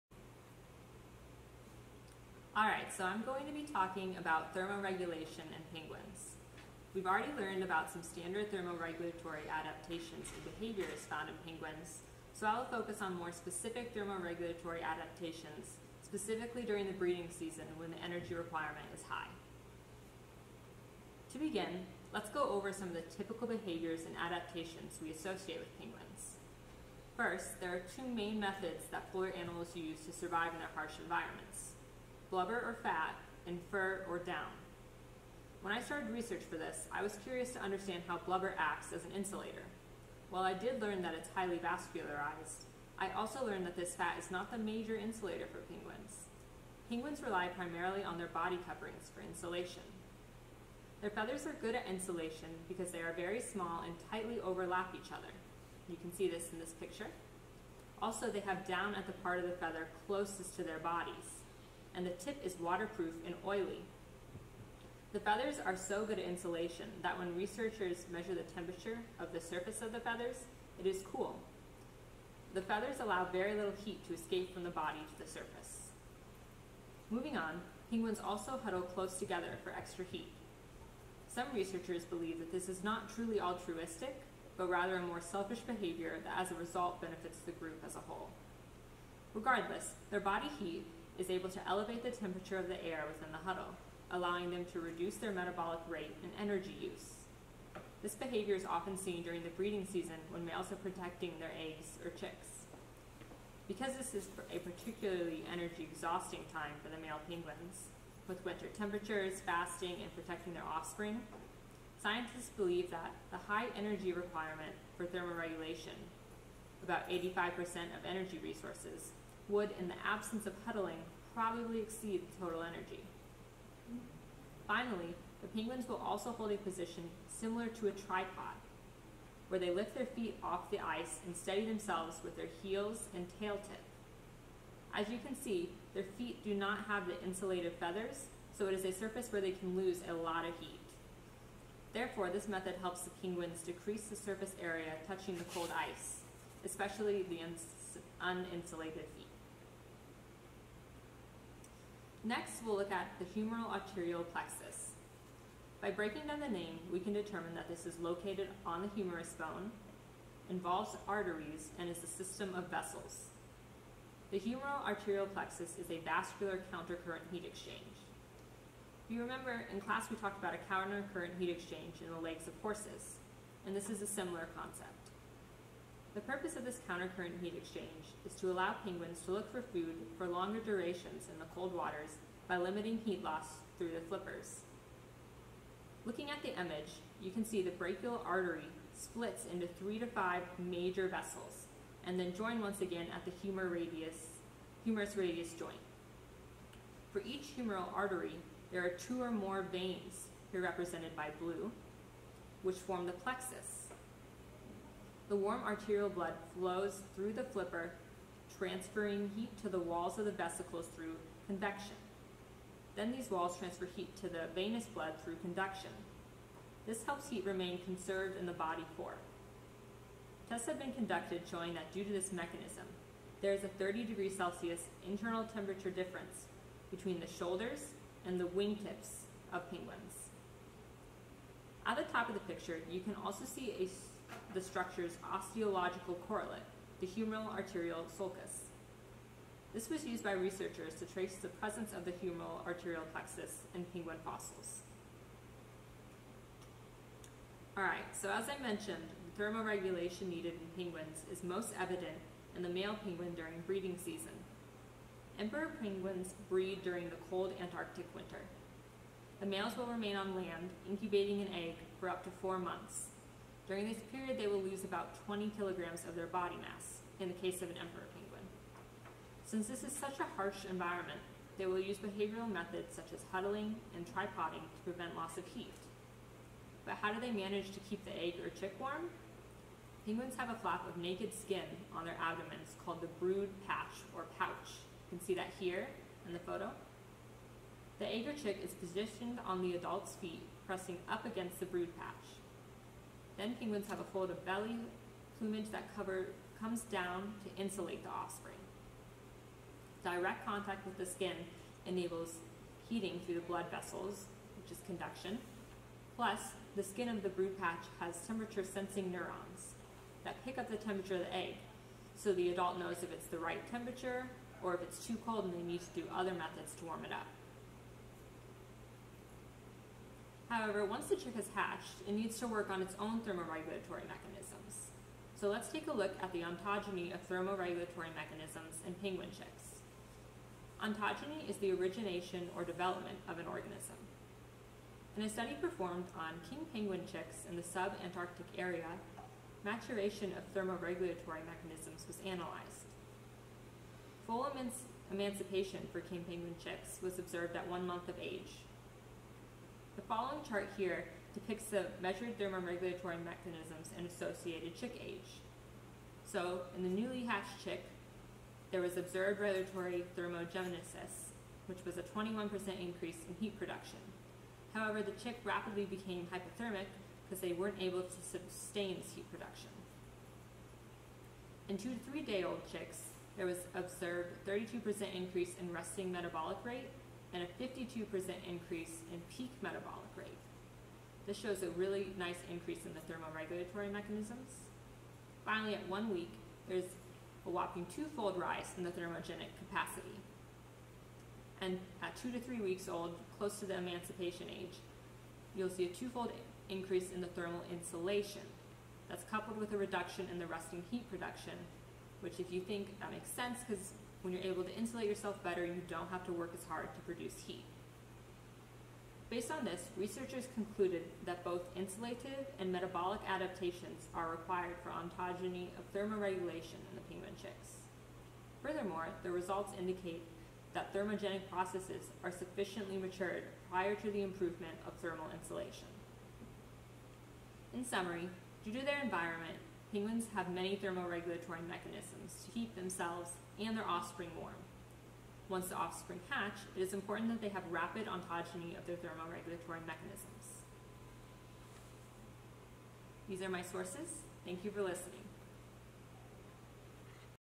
Thermoregulation in Penguins
Discussion of mechanisms used by penguins to avoid hypothermia.